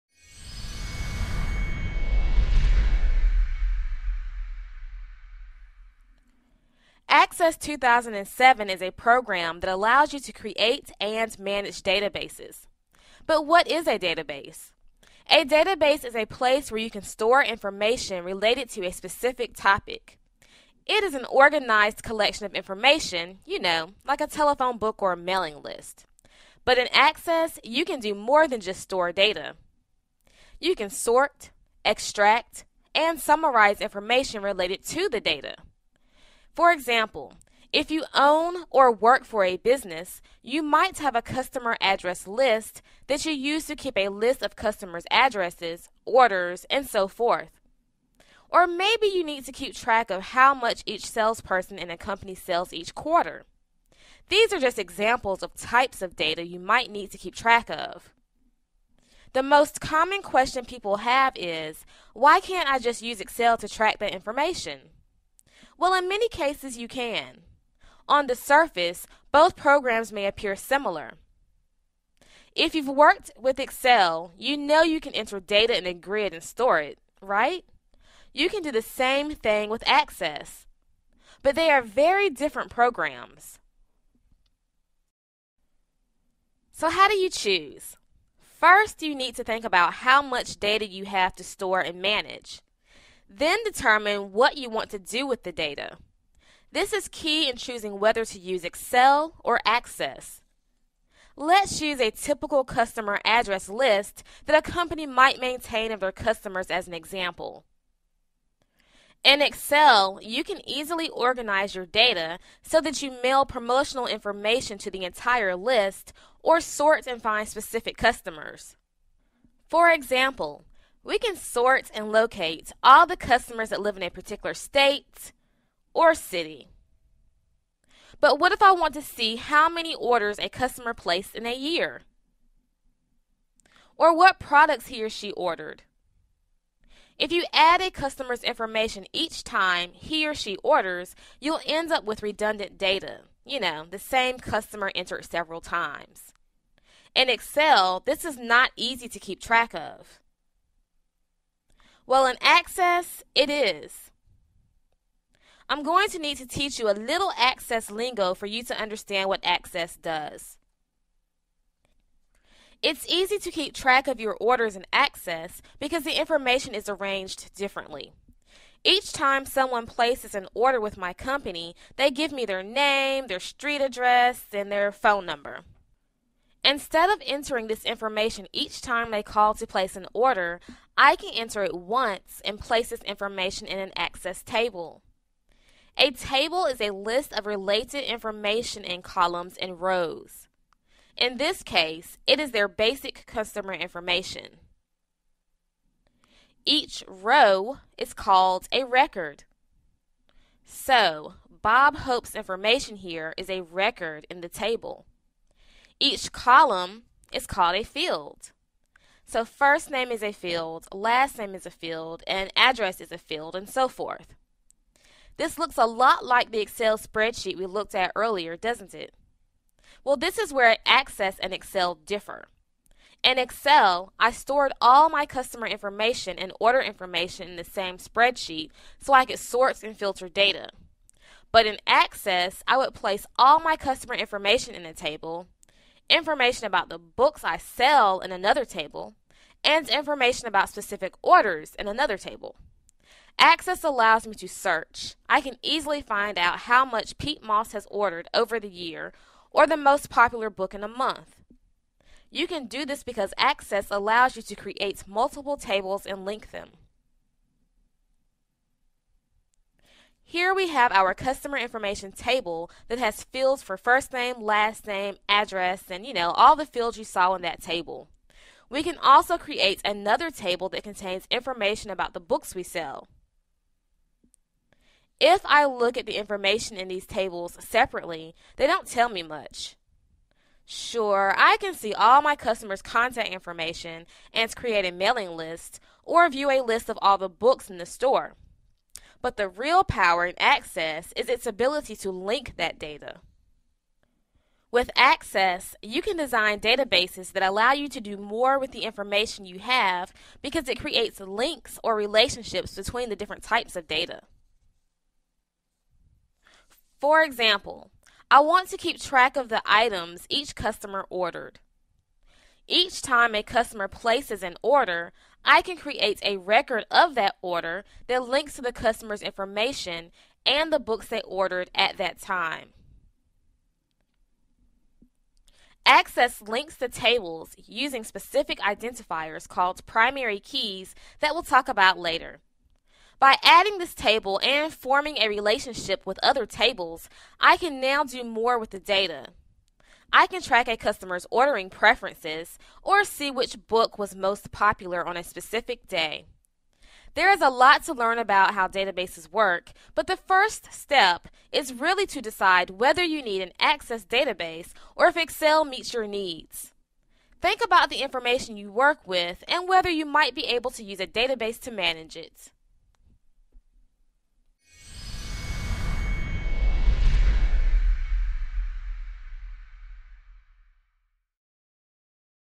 Access 2007: Why Do I Need a Database?
In this video, you’ll learn more about why you'd need an Access 2007 database. for our text-based lesson.

This video includes information on:
• What a database is useful for
• Entering data in Access 2007